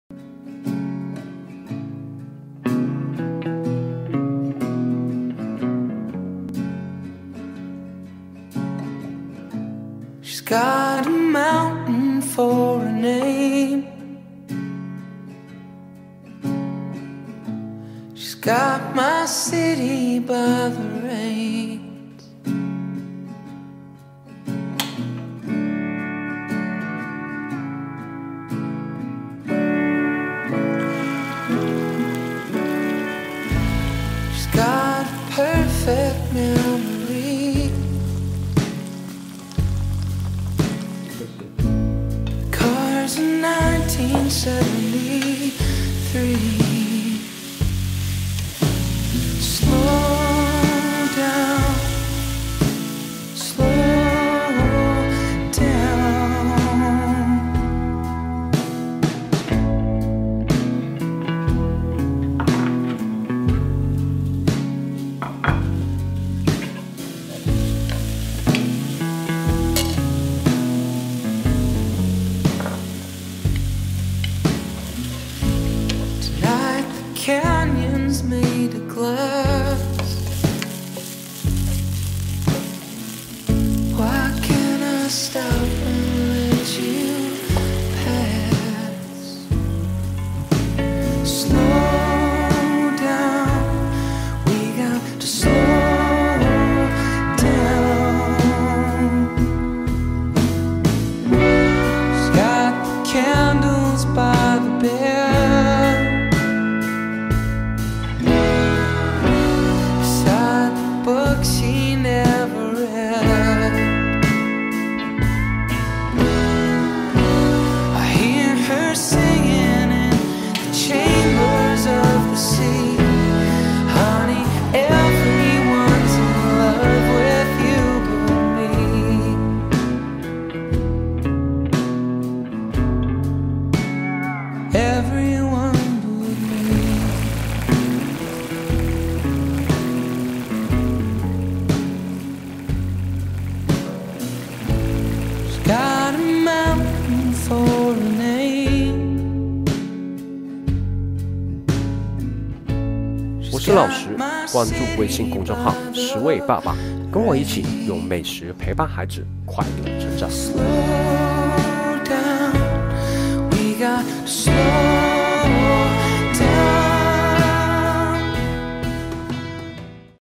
【肉末茄子煲】家常美食来一份，无需重油重盐重调味也可以很好吃哒！【拾味爸爸】
今天分享的这道家常美食估计大家都吃过了，好吃又下饭。

如果一顿饭有了它，全家人都会吃得特别香特别满足。当然了，这道菜之所以这么有“杀伤力”，也和调味重滋味浓有关系。

不过重油重盐重调味可是非常不健康的，所以我特意做了个改良版，让全家老少都能开心品尝这道菜，而且还可以确保营养健康哦~
参考月龄：2岁及以上